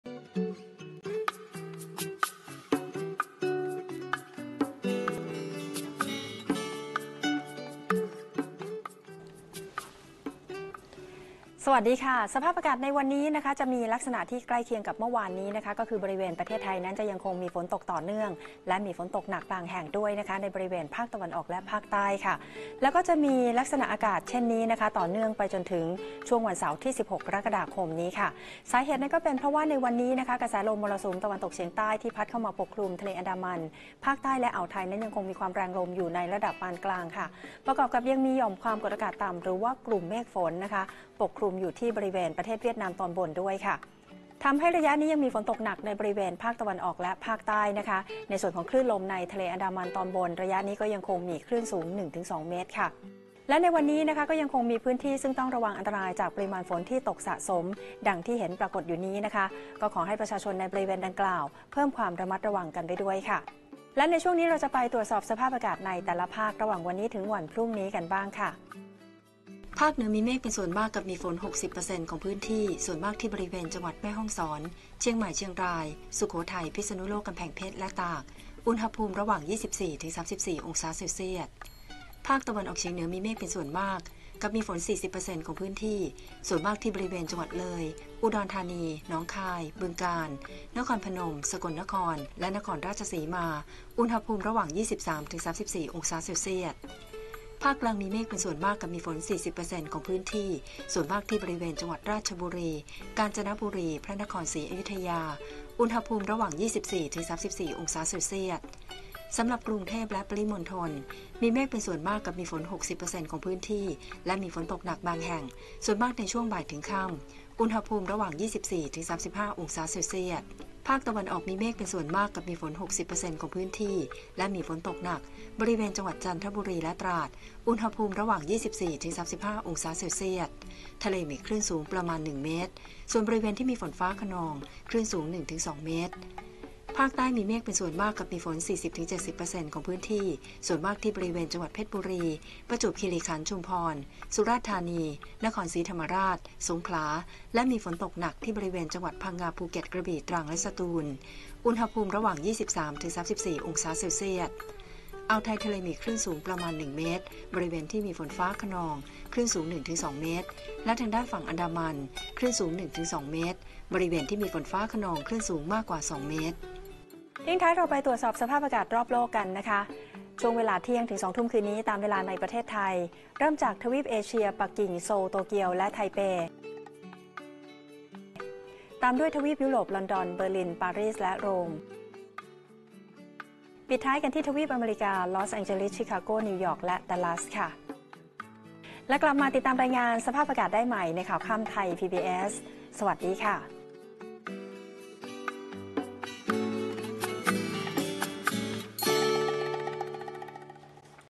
กทม.มีฝนช่วงบ่าย-ค่ำ 150759
ประเทศไทยยังคงมีฝนตกต่อเนื่องกับมีฝนตกหนักบางแห่ง และยังมี 6 จังหวัดต้องระวังอันตรายจาก ปริมาณฝนที่ตกสะสม กรุงเทพฯ-ปริมณฑลยังมีฝนตกหนักในช่วงบ่ายถึงค่ำ